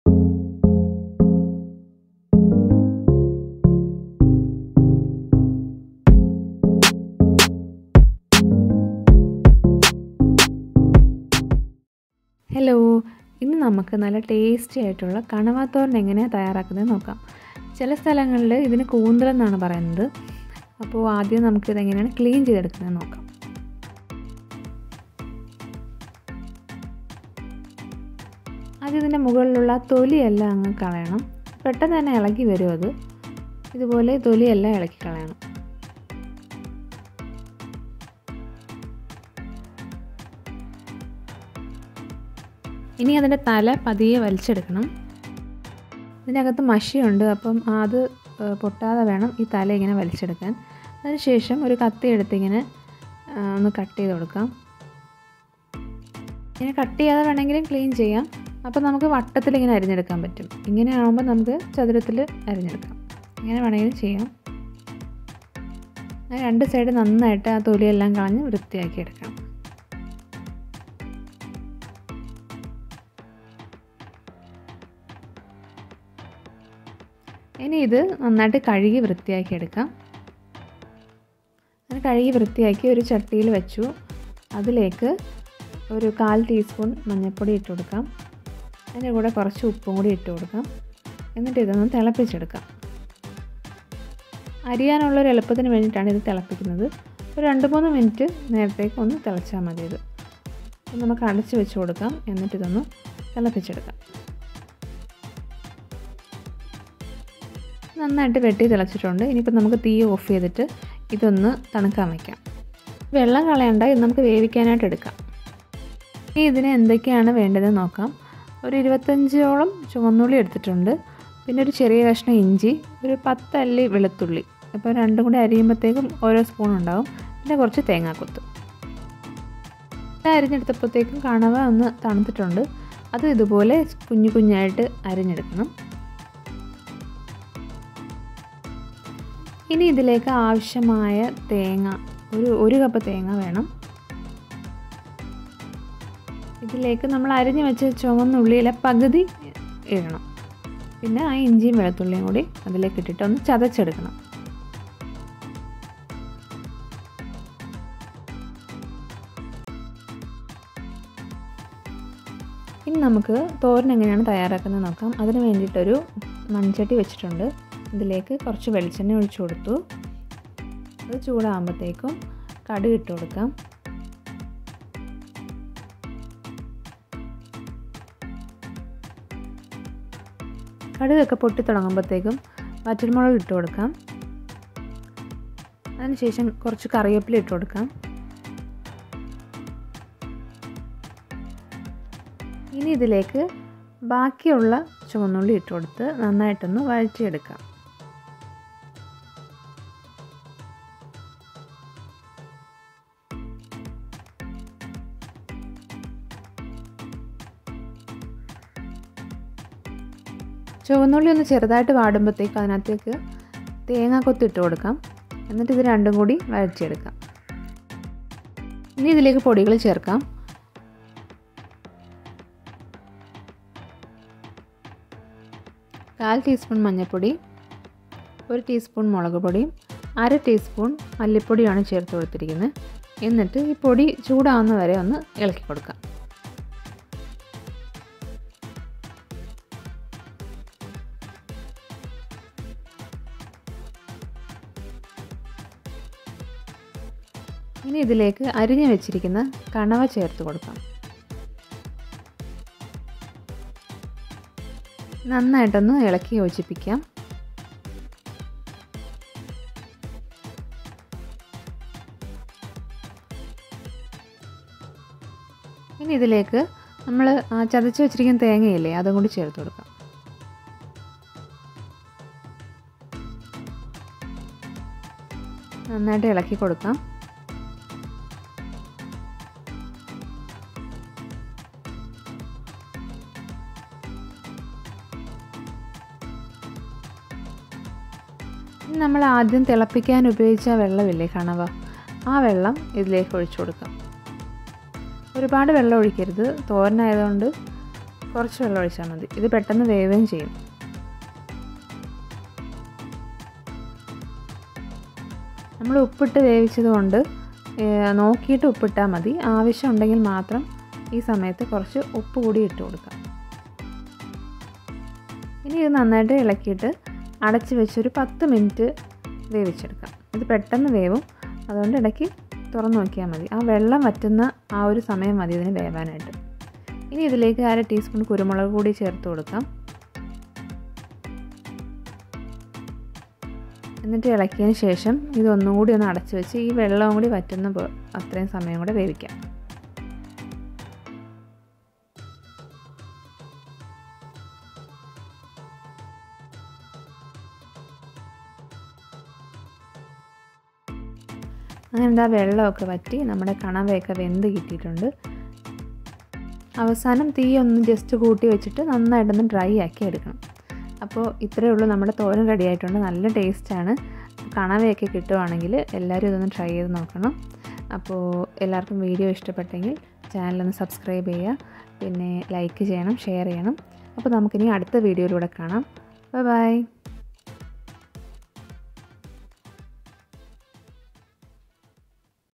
Kanava thoran | Koonthal thoran | Squid recipe | കണവ തോരൻ Kerala style tasty side dish | Have A Luk
Hi friends, I am Hima

Welcome to Have A Luk

Do try this recipe and please let me know the feedback through your valuable comments.